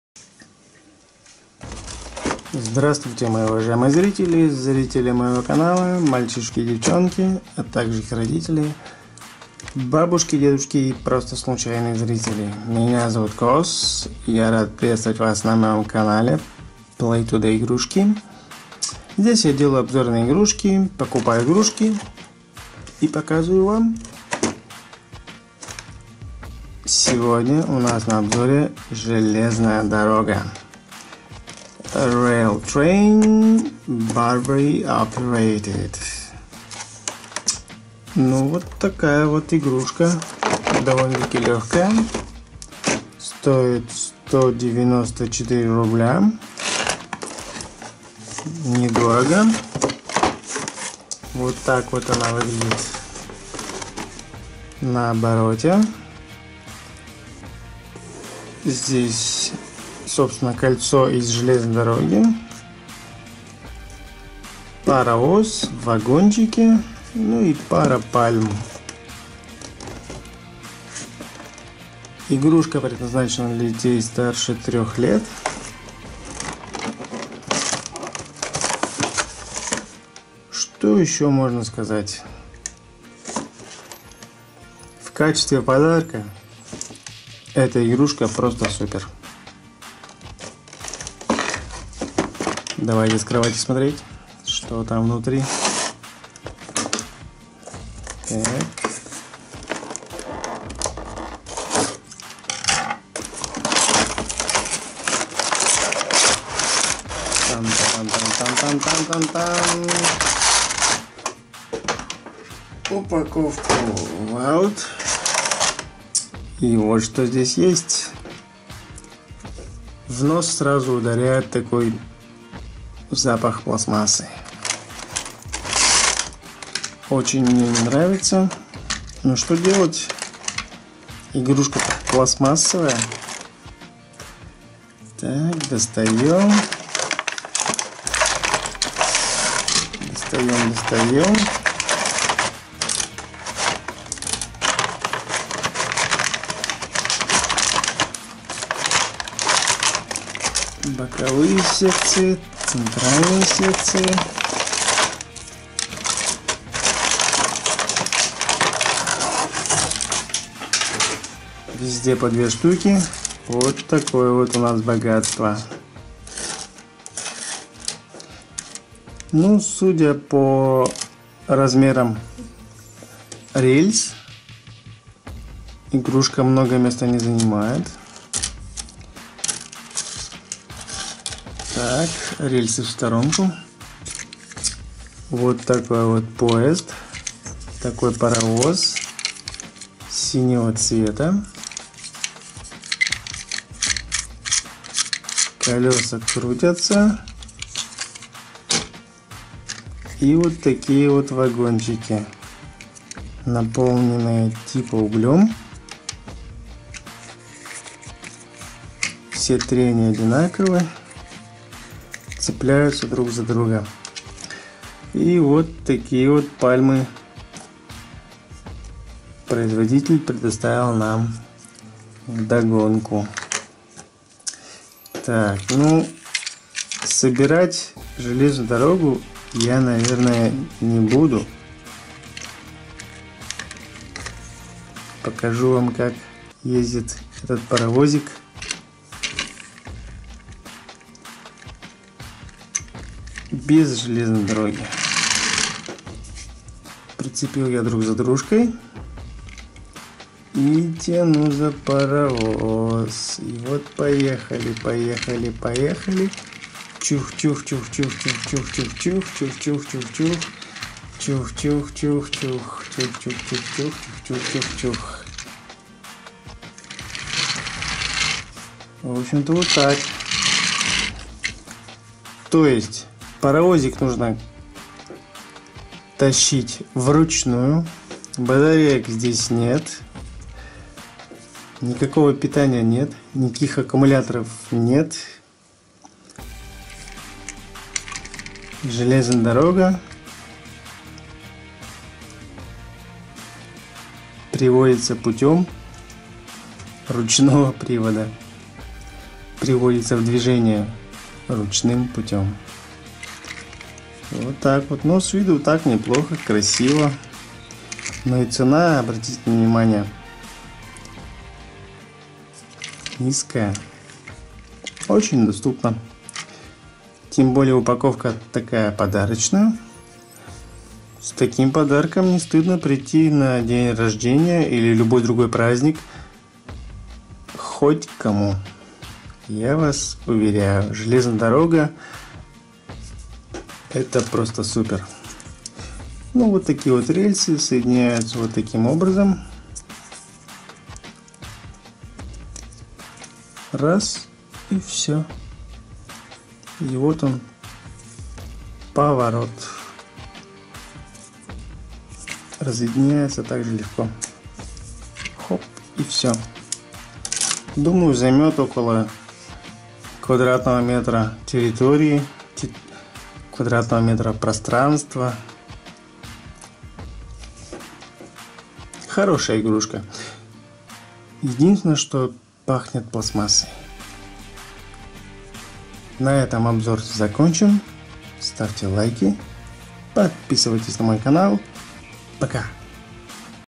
Обзор игрушек. Железная дорога "Super Station Set."
Железная дорога "Super Station Set" - это игрушка для детей старше 3-х лет.